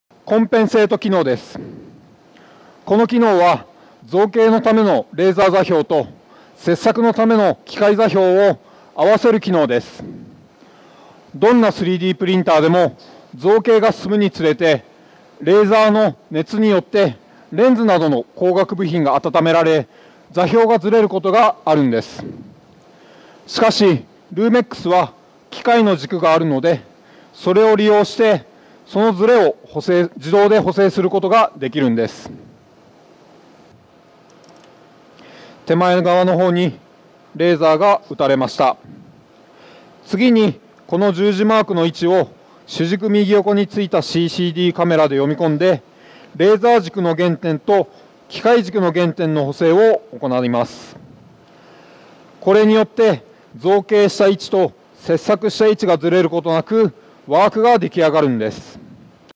The compensate function of LUMEX
One noteworthy advantage to have a spindle on a 3D metal printer is its compensation capability using its mechanical coordinates.
As a result, it can reduce an error to minimum between command and actual laser processing spot.